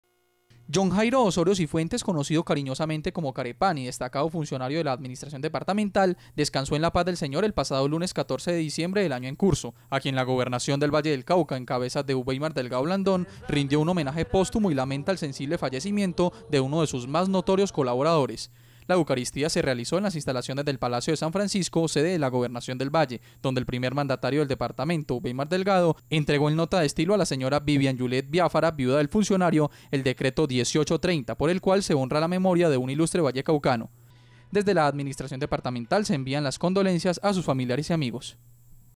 Gobernación del Valle del Cauca rinde homenaje póstumo a uno de sus más destacados funcionarios
Santiago de Cali, 16 de Diciembre de 2015. Jhon Jairo Osorio Cifuentes conocido cariñosamente como “Care Pan” y destacado funcionario de la administración departamental, descansó en la paz del señor el pasado lunes 14 de Diciembre del año en curso, a quien la Gobernación del Valle del Cauca en cabeza de Ubeimar Delgado Blandón, rindió un homenaje póstumo y lamenta el sensible fallecimiento de uno de sus más notorios colaboradores.

La eucaristía se realizó en las instalaciones del Palacio de San Francisco sede de la Gobernación del Valle, donde el primer mandatario del departamento Ubeimar Delgado entregó en nota de estilo a la señora Vivian Julieth Viáfara, viuda del funcionario, el decreto 1830 por el cual se honra la memoria de un ilustre vallecaucano.

Desde la administración departamental se envían las condolencias a sus familiares y amigos.